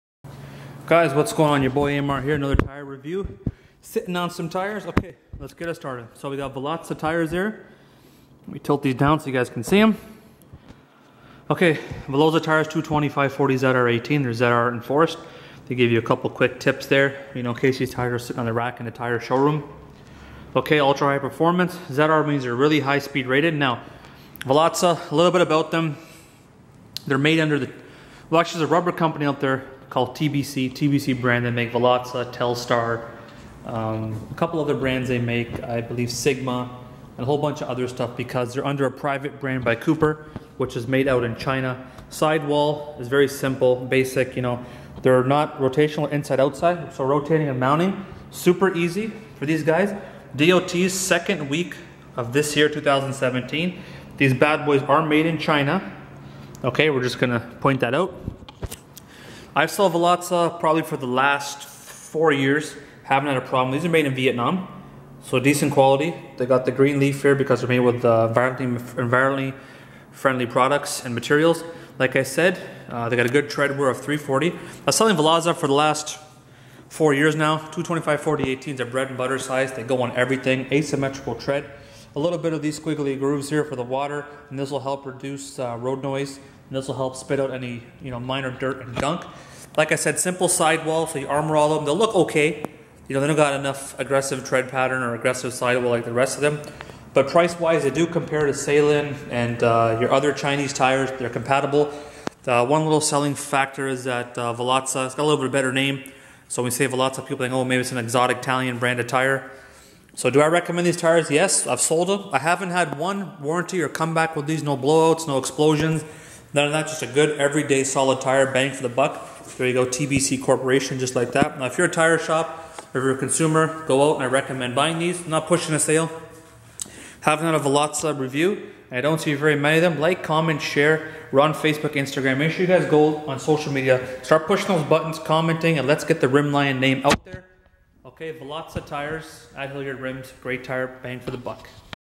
VELOZZA TIRE ZXV4 REVIEW (SHOULD I BUY THEM?)
WELCOME BACK RIM LIONS! SO TODAY I DID ONE OF MY FAMOUS ON THE RACK TIRE REVIEWS! SO TODAY WE HAVE A VELOZZA ZXV4 PERFORMANCE TIRE! THE SIZE IS P225/40/18.  SO THESE TIRE IS PRIVATE BRAND TIRE MADE UNDER THE COOPER FAMILY! UNDER TBC BRANDS, OR TBC CORPORATION ETC. SO A GREAT OVERALL TIRE FOR THE VALUE! I HAVE BEEN SELLING THESE FOR OVER 4 YEARS NOW AND HAVEN'T HAD ANY ISSUES WITH THEM AT ALL! NO COMEBACKS, WARRANTY ISSUES OR BLOW OUTS! GREAT TIRE! WATCH THE REVIEW AND HAVE LOOK! CHECK OUR OUR OTHERS!